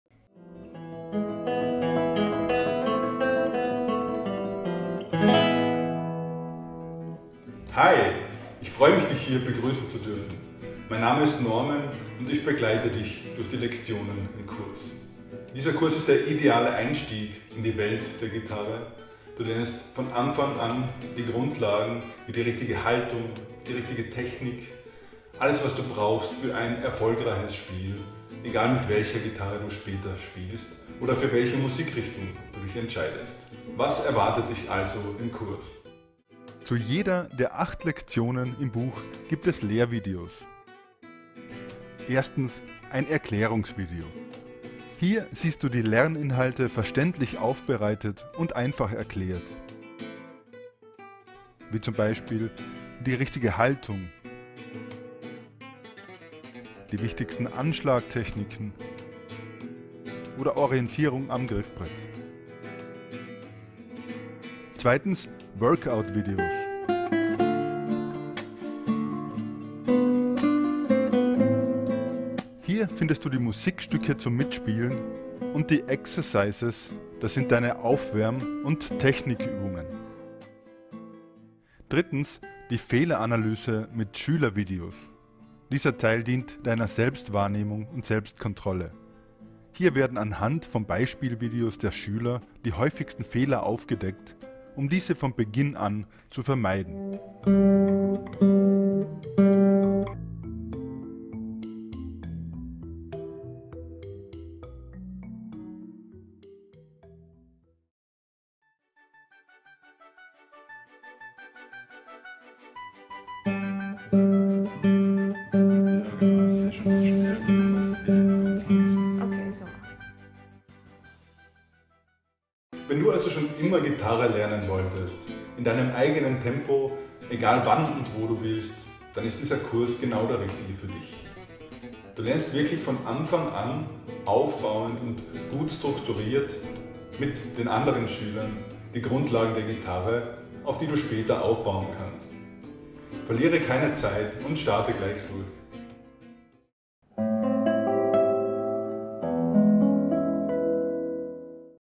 Gitarre lernen online - von Grund auf Klassische Gitarre lernen (Gitarrenschule + Online-Kurs)
Erfülle dir deinen Traum und lerne endlich Gitarre!
Diese Methode wurde entwickelt, damit du 99% der häufigsten Fehler an der Gitarre von Anfang an vermeidest! Klassische Gitarre für Erwachsene.
Der Kurs vermittelt die Grundkenntnisse an der Gitarre, nach Kriterien der Musikschulen (KOMU Lehrplan).

alle Musikstücke und Lerninhalte im Buch sind in mehr als 65 Videos verpackt (inkl. Schülervideos mit den häufigsten Fehlern an der "Klassischen Gitarre").
Das Buch zum Onlinekurs ist eine eigenständige Gitarrenschule.

"Der Elementarkurs gilt als idealer Einstieg in die Welt der Gitarre und vermittelt die wichtigsten Grundkenntnisse der “klassischen” Methode (Fingerpicking, Fingerstyle), auf eine moderne, zeitgemäße Art.

Bei der Gestaltung des Kurses wurden Überlegungen angestellt, wie Lerninhalte sinnvoll online vermittelt werden können, um den Schülern eine fundierte Online-Ausbildung zu ermöglichen, die mit einer Ausbildung an einer Musikschule vergleichbar ist.

Der Kurs ist aufbauend gestaltet und schafft somit die besten Voraussetzungen für ein erfolgreiches Spiel, egal mit welcher Art von Gitarre später gespielt werden soll oder für welche Musikrichtung man sich in der Zukunft entscheidet."